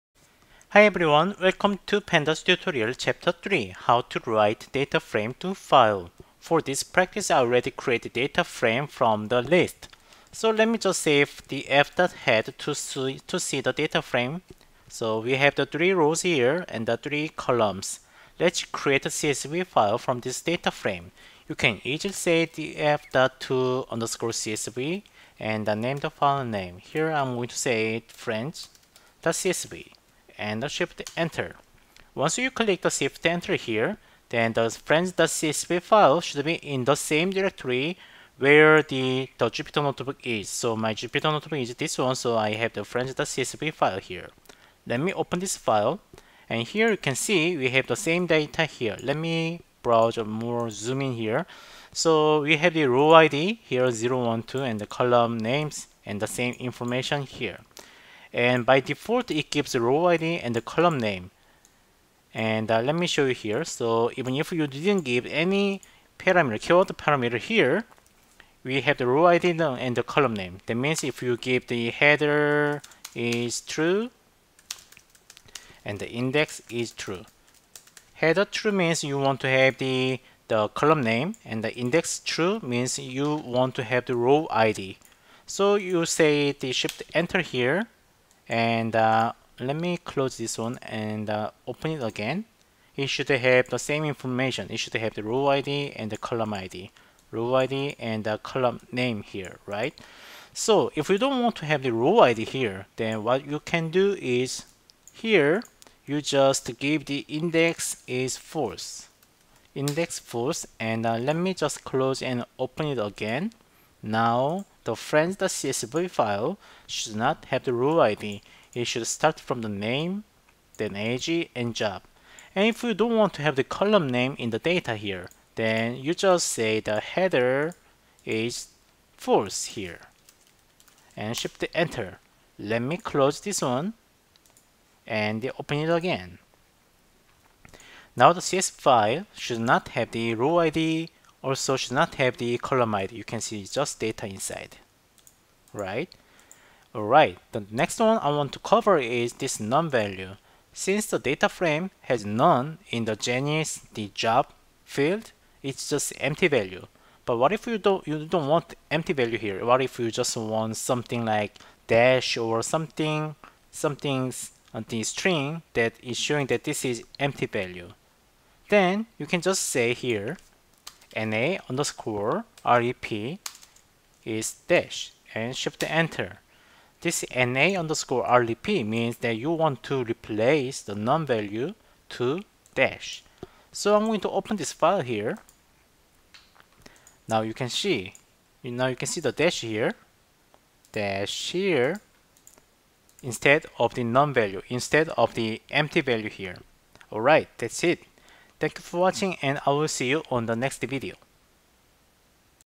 [Pandas Tutorial] How to write csv file from dataframe (to_csv)
you can practice yourself by downloading below jupyter notebook

In this video, I show how to create csv file from dataframe